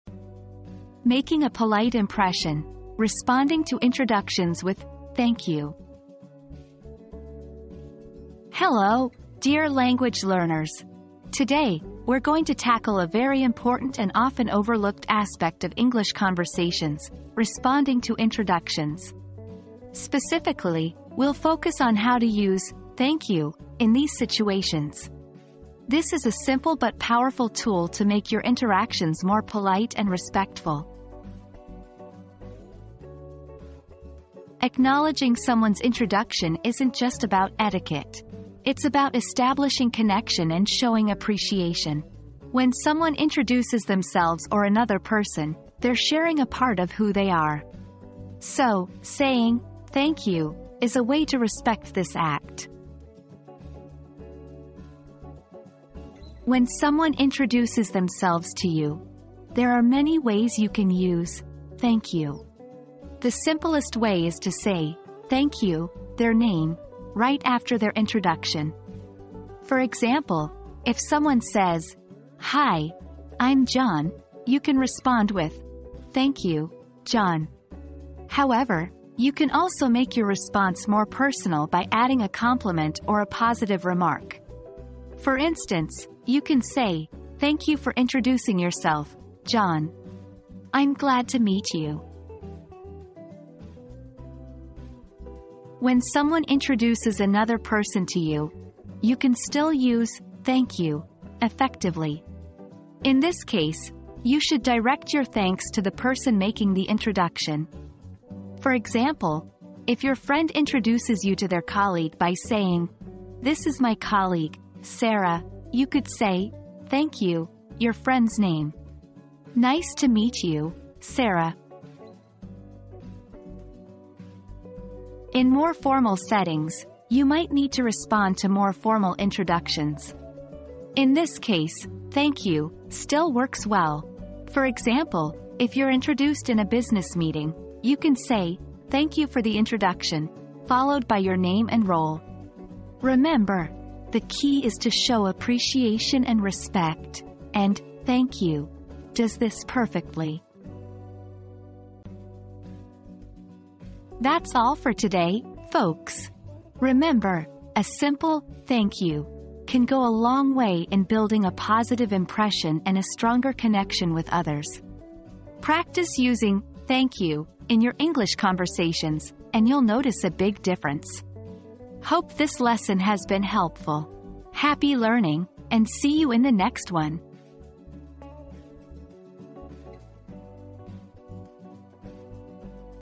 Making a Polite Impression: Responding to Introductions with 'Thank You'
Polite English Conversations: Using 'Thank You' to Respond to Introductions • Learn how to make a great first impression in English conversations by using 'Thank You' to respond to introductions. This simple but powerful tool shows respect and appreciation, and can be personalized to make your interactions even more polite. Follow these tips for responding to self-introductions, introductions of others, and more formal introductions.

00:00 • Introduction - Making a Polite Impression: Responding to Introductions with 'Thank You'
00:34 • Why 'Thank You' Matters
00:55 • Responding to Self-Introductions
01:35 • Responding to Introductions of Others
02:04 • Acknowledging More Formal Introductions